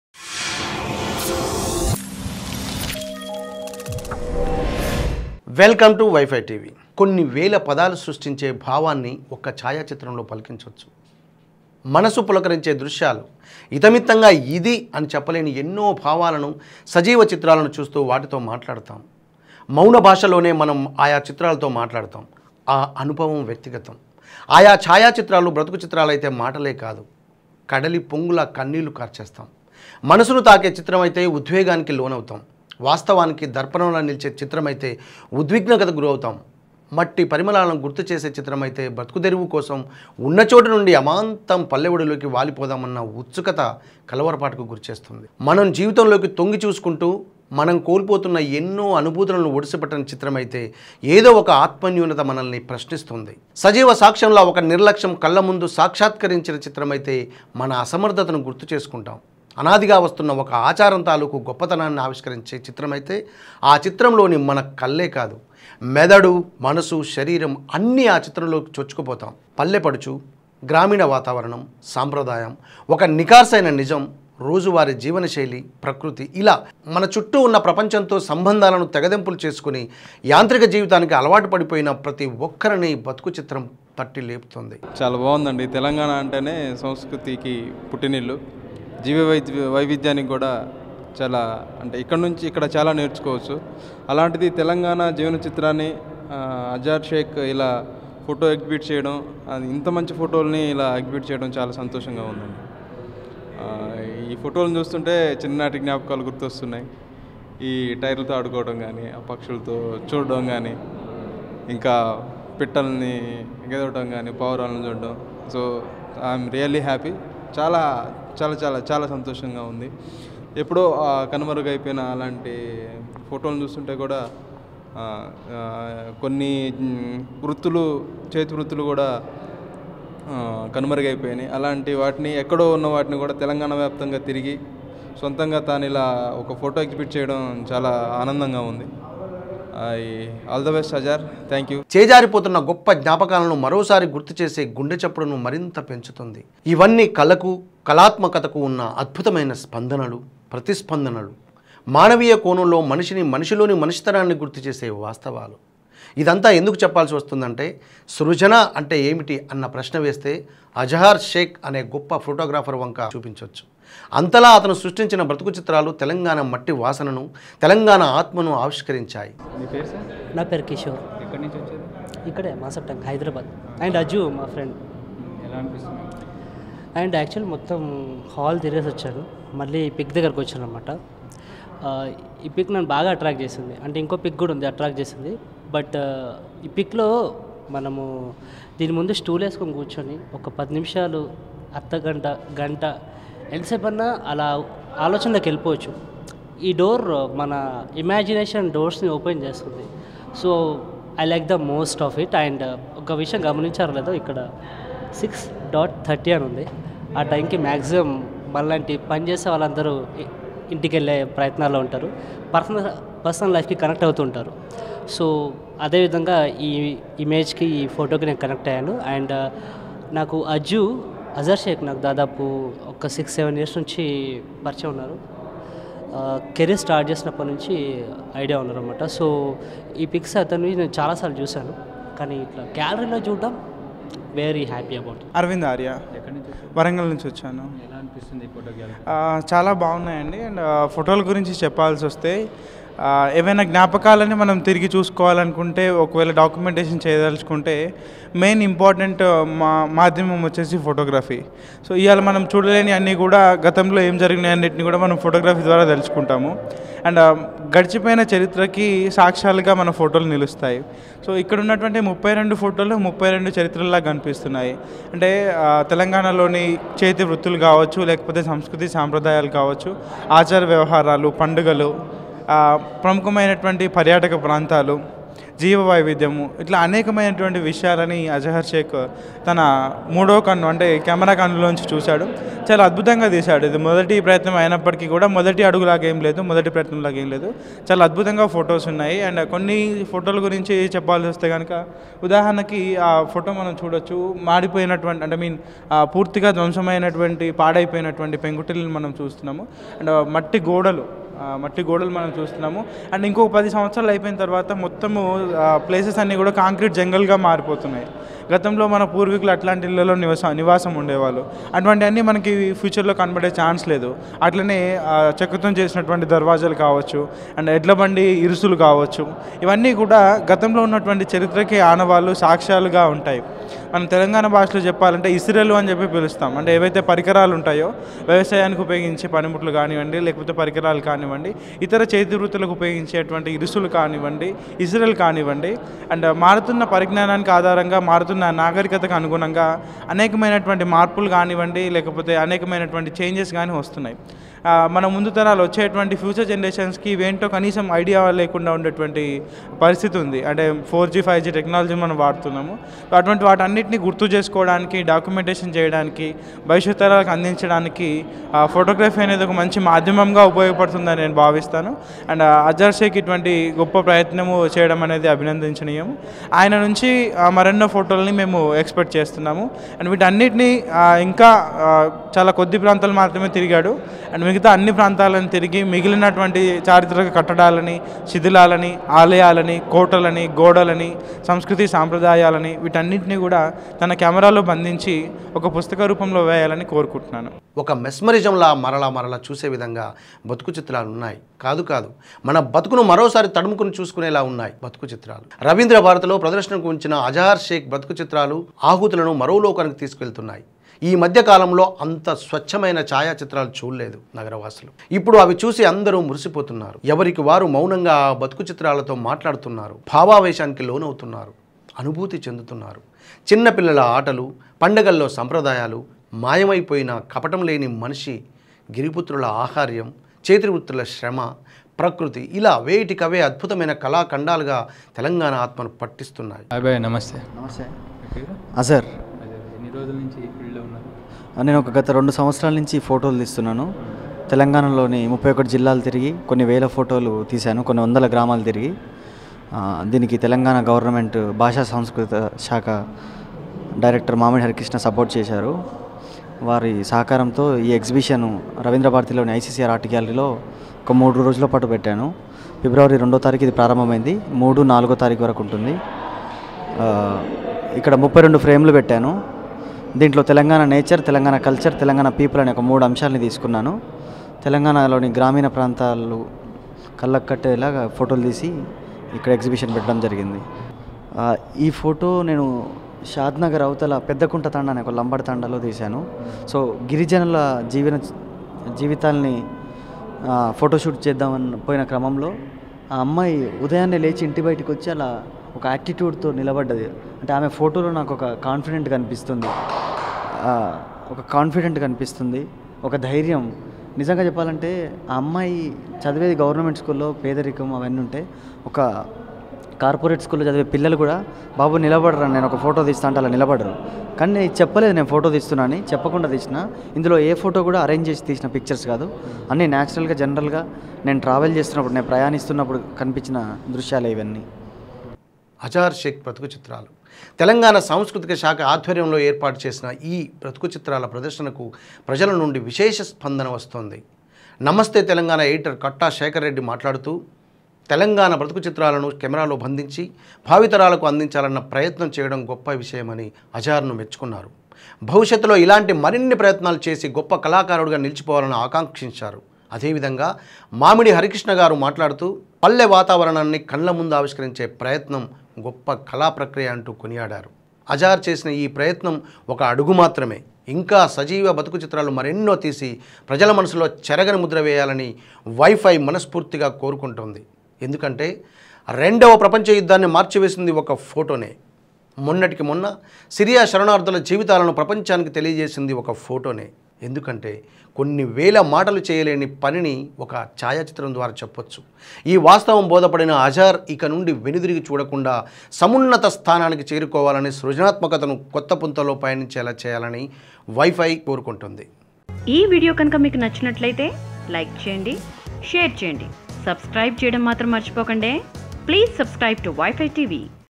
TELANGANA BATHUKU CHITRAM Photo Exibition by Azhar Sheik @ Ravindra Bharati | Y5 tv
Azhar Sheik is a well known photographer who is an expert in capturing the liveliness of the nature. not only the nature but also he cature the lives of the telangana people naturally. his photos speak volumes about the life and culture. the photo exibition at ravindra bharati by azhar sheik shows the real culture and traditions of telangana.